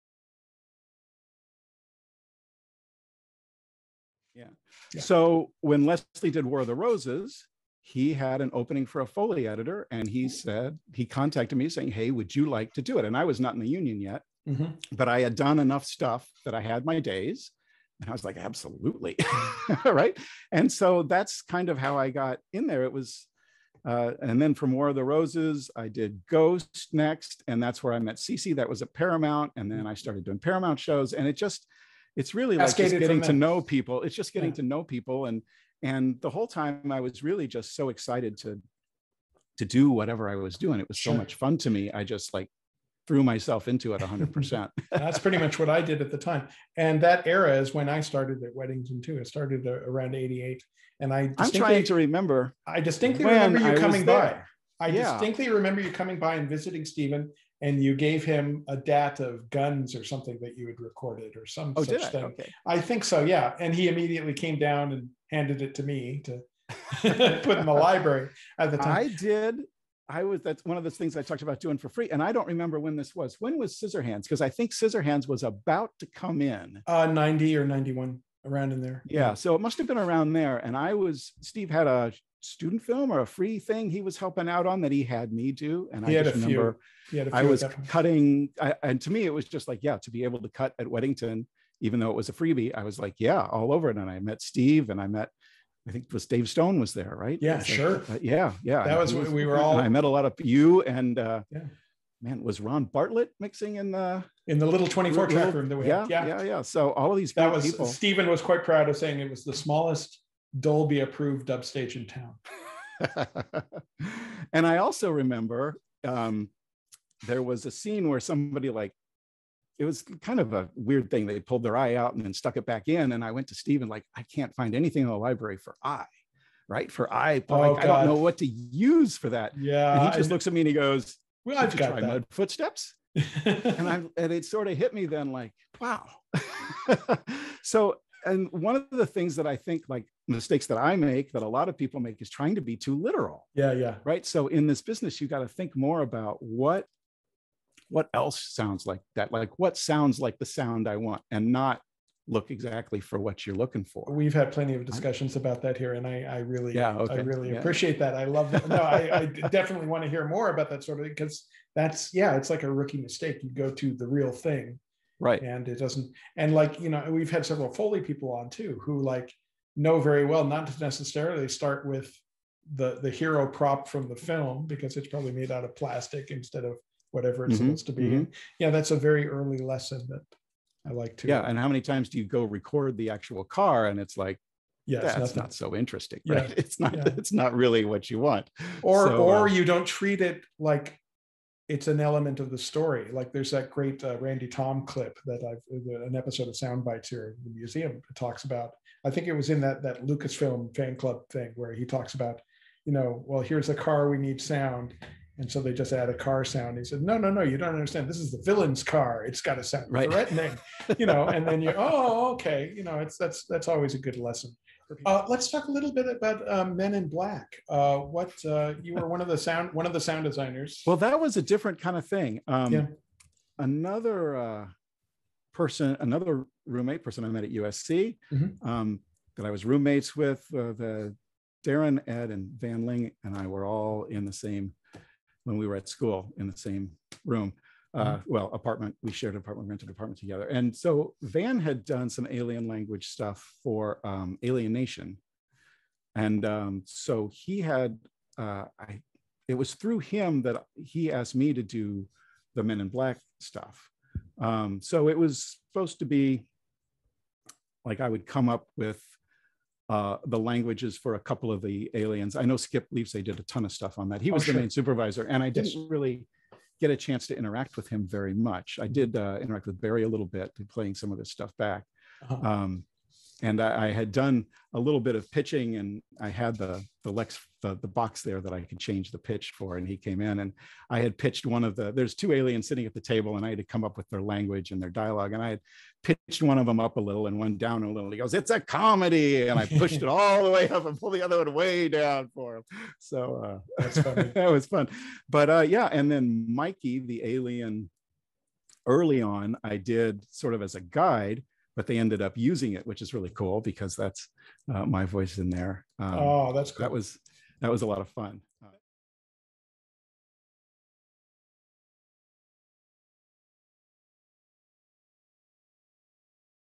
Hollywood Sound Museum Webinar #37 Highlights - Chuck Michael - 7/11/2021
Clips from Steve Lee's chat with Sound Editor Chuck Michael about starting his career in film sound, and his work on "Men in Black."